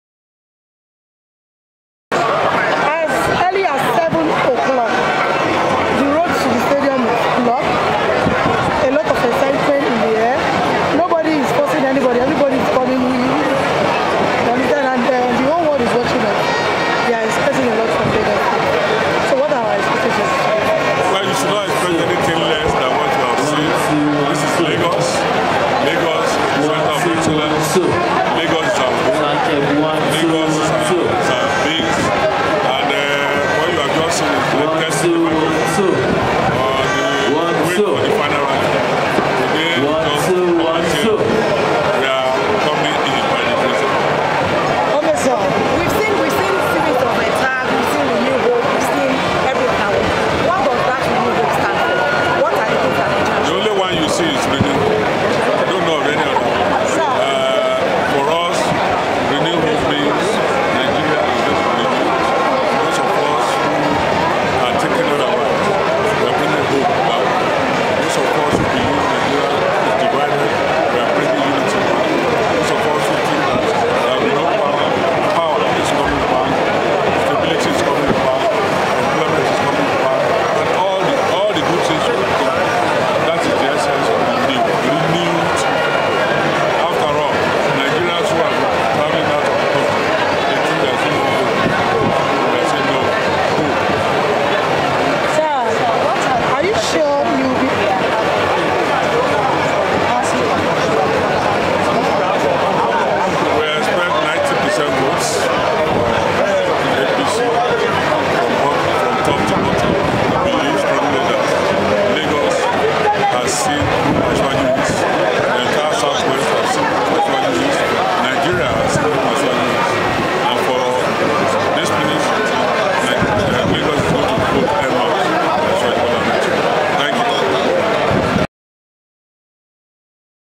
2022 APC Presidential Rally in Lagos. Hon. James Abiodun Faleke
2022 APC Presidential Rally in Lagos:  Hon. James Abiodun Faleke (Member Federal House of Representatives / Secretary APC Presidential Campaign Council) gave his confidence in coming 2023 general election with the great turn out at the rally.